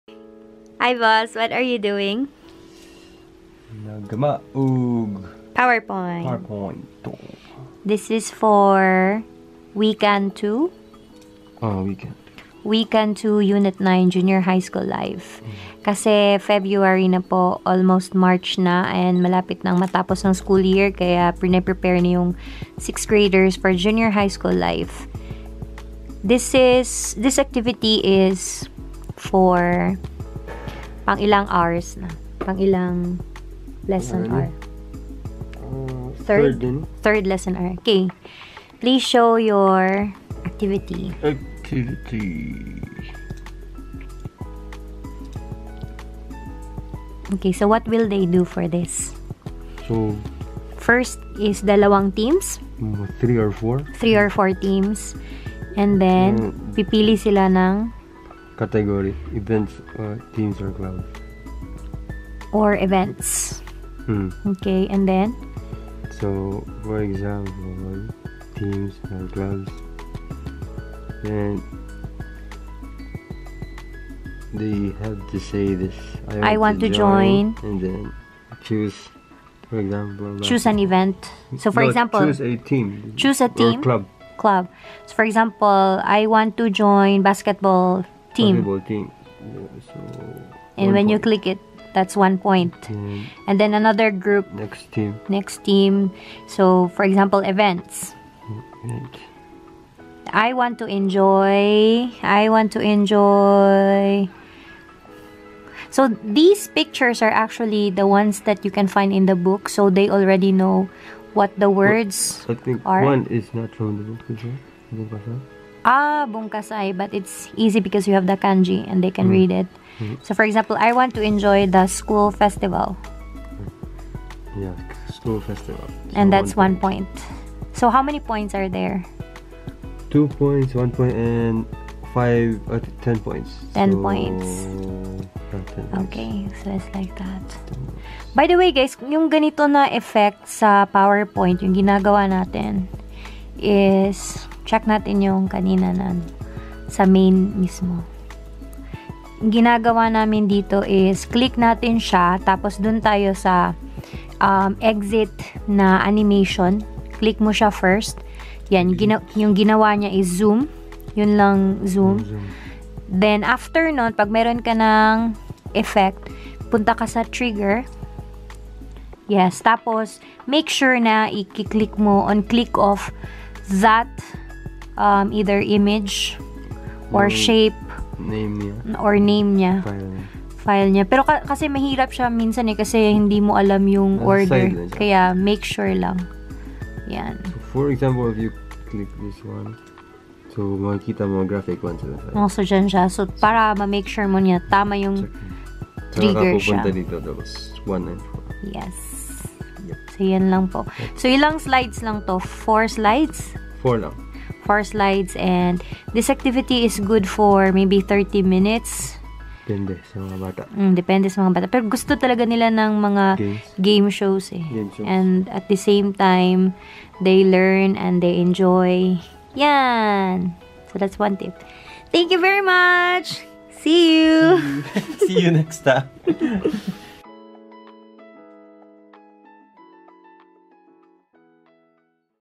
WeCan2 6th Grade ES Unit 9 Activity using PowerPoint | for Assistant Language Teachers
Here's another activity for 6th Grade ES (WeCan2 Unit 9 3rd Hour) using PowerPoint.